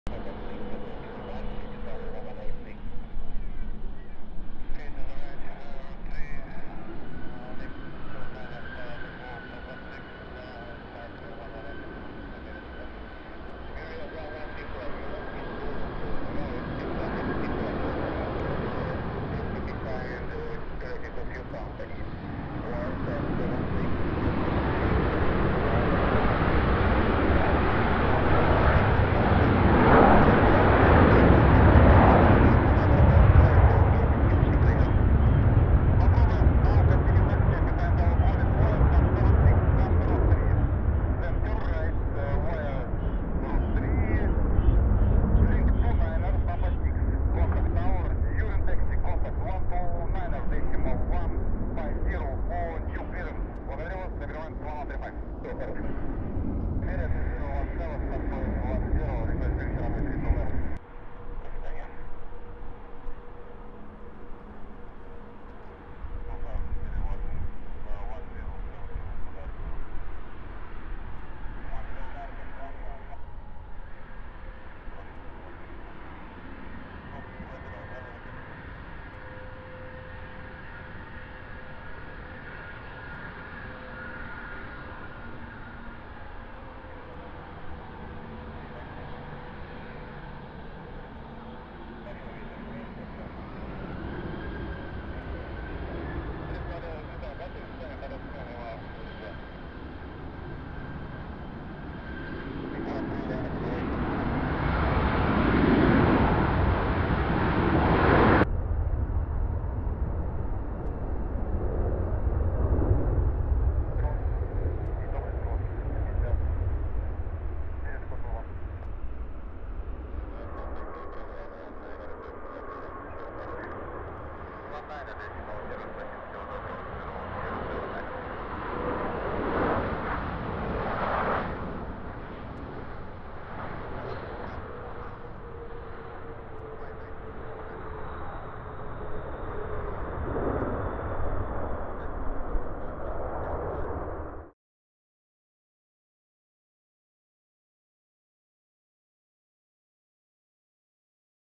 Взлетают "Суперджеты". А/к "Ямал" (борт Герой России Роман Филипов) и "Иркутские авиалинии". DME.
Взлетают "Суперджеты". А/к "Ямал" (борт Герой России Роман Филипов) и "Иркутские авиалинии". Снято в Домодедово, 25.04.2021.
Буквально пару лет назад самолёт "Сухой Суперджет" в России можно было увидеть только в ливрее "Аэрофлота" и очень редко у кого-то ещё. В 2021 году число произведенных SSJ-100 перевалило уже за 200 штук и этими самолётами постоянно пополняется флот многих российских авиакомпаний, в том числе "Ямал" и "Иркутские авиалинии". В это видео попал борт с именем Героя России Романа Филипова, погибшего в Сирии военного лётчика.